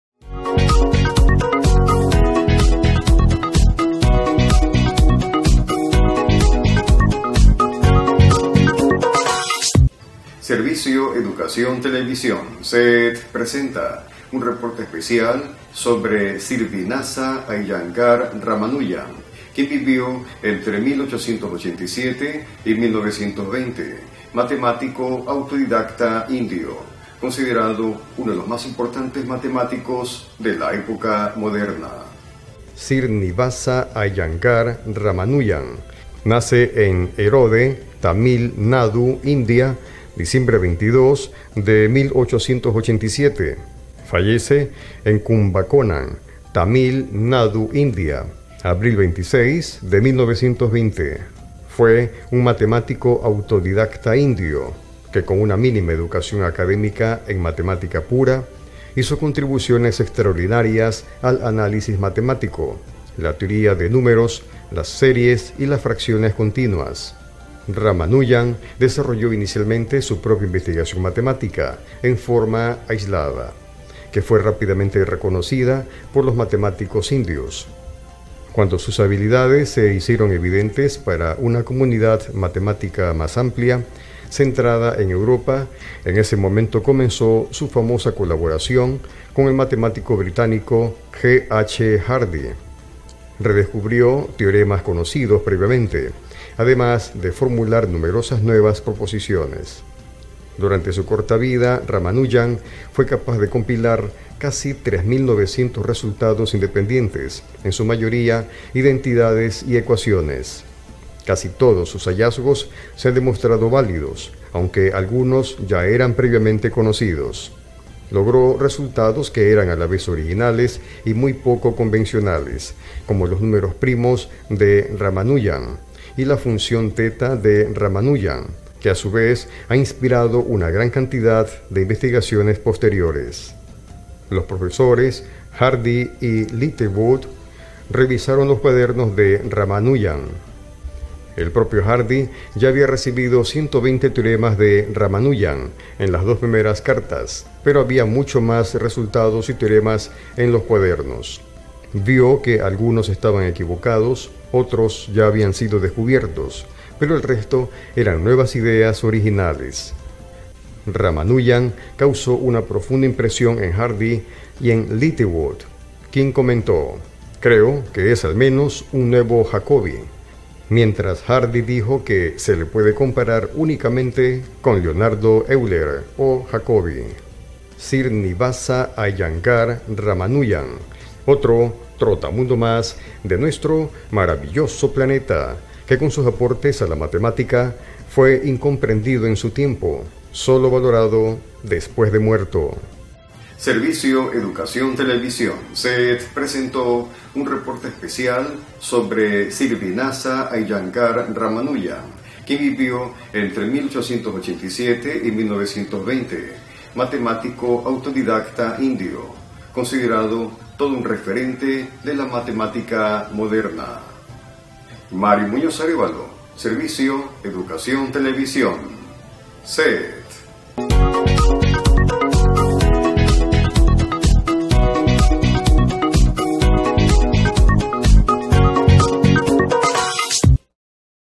Homenaje a Srinivāsa Aiyangār Rāmānujan al 2.020
Amigos universales de Servicio Educación Televisión (SET), ahora les comparto un reporte especial sobre Srinivāsa Aiyangār Rāmānujan, matemático autodidacta indio, aportó al análisis matemático, la teoría de números, las series y las fracciones continuas, que el día de hoy (martes, diciembre 22 del 2.020), se recuerda su natalicio. Esperamos que sea de vuestro interés cultural. Saludos cordiales.